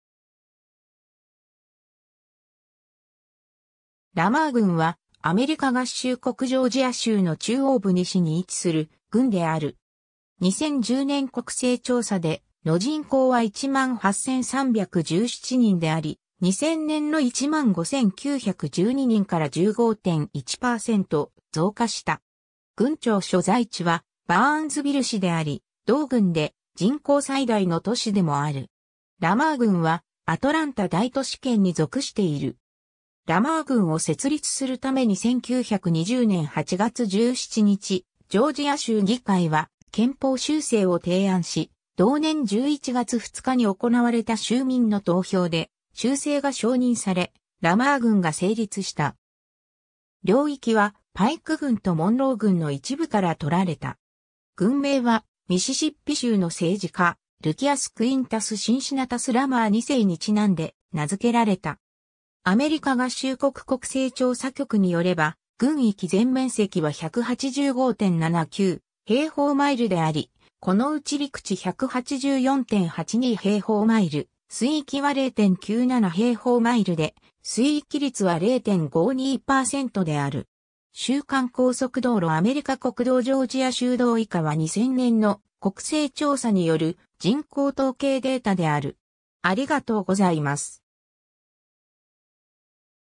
ラマー郡 (ジョージア州)
ラマー郡（英: Lamar County）は、アメリカ合衆国ジョージア州の中央部西に位置する郡である。
2010年国勢調査での人口は18,317人であり、2000年の15,912人から15.1%増加した。
郡庁所在地はバーンズビル市（人口6,755人）であり、同郡で人口最大の都市でもある。
 ラマー郡はアトランタ大都市圏（正確にはアトランタ・サンディスプリングス・マリエッタ大都市統計地域）に属している。
 ラマー郡を設立するために1920年8月17日、ジョージア州議会は憲法修正を提案し、同年11月2日に行われた州民の投票で修正が承認され、ラマー郡が成立した。
領域はパイク郡とモンロー郡の一部から取られた。
郡名はミシシッピ州の政治家ルキアス・クインタス・シンシナタス・ラマー2世に因んで名付けられた。
 州間高速道路 アメリカ国道 ジョージア州道 以下は2000年の国勢調査による人口統計データである。